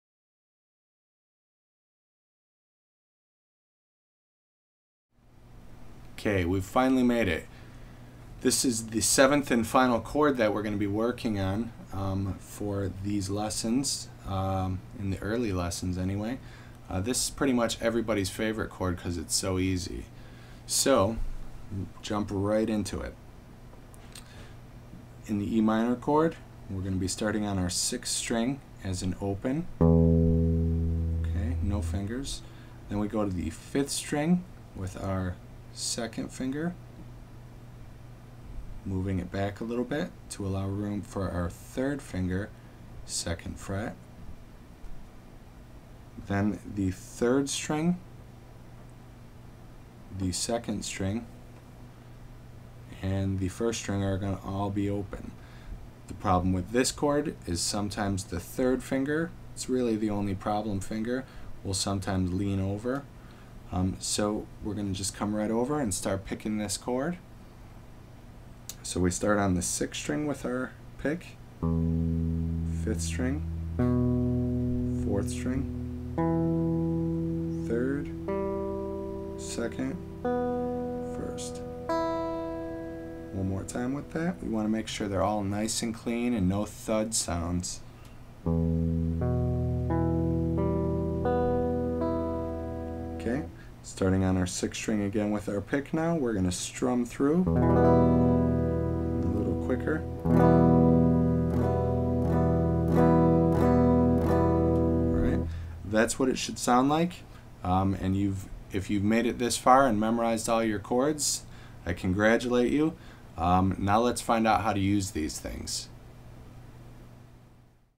12. Level 2 - Chords: Play the Em Chord
Learn to play the 'E' minor chord in this frwee guitar lesson video